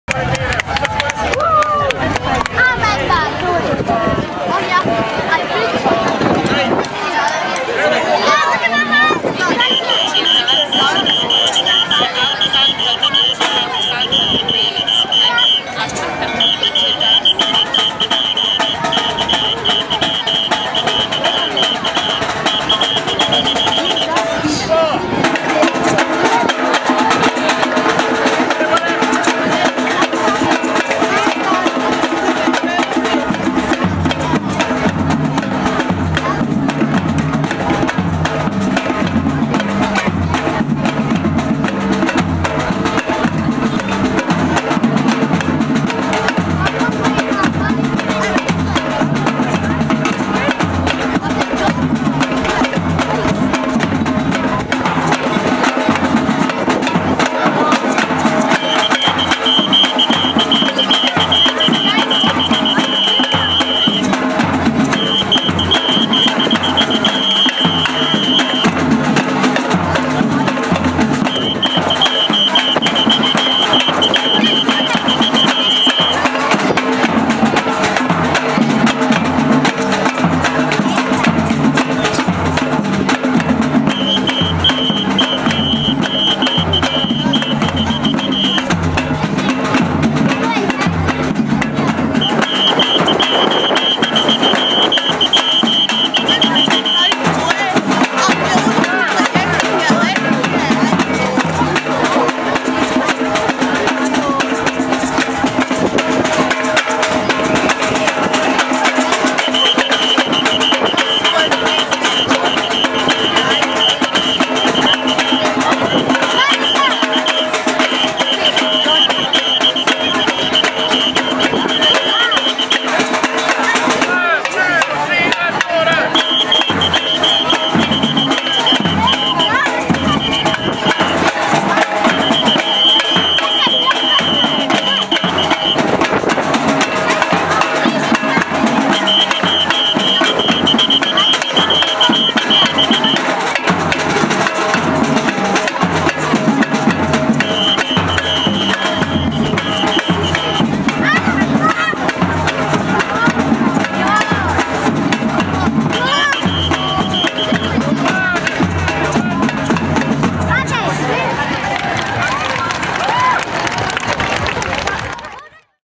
Youth Drumming Group - Lochgelly Gala 2015
Youth drumming group performing at the Lochgelly Gala 2015. Members are from the communities of Cowdenbeath and Cardenden.